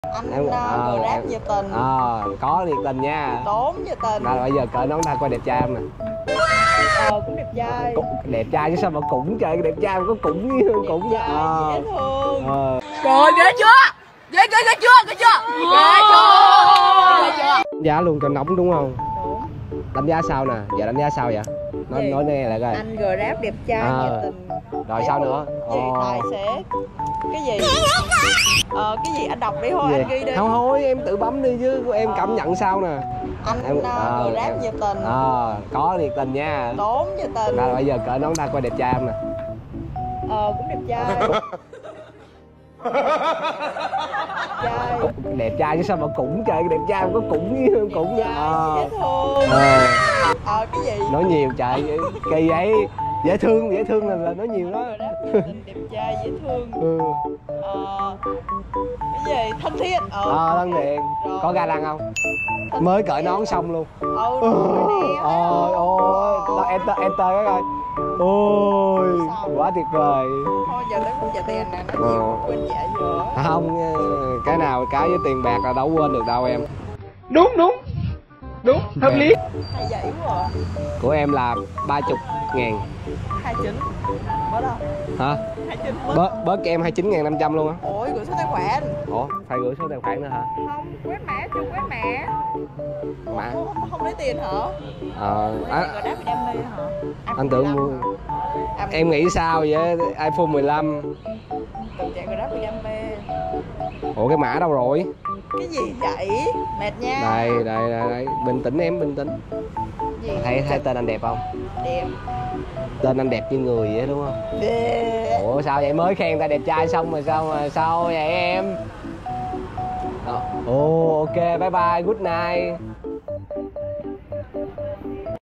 Anh grab may mắn gặp được em khách siêu cute và cái kết!
Anh grab gặp được em khách dễ thương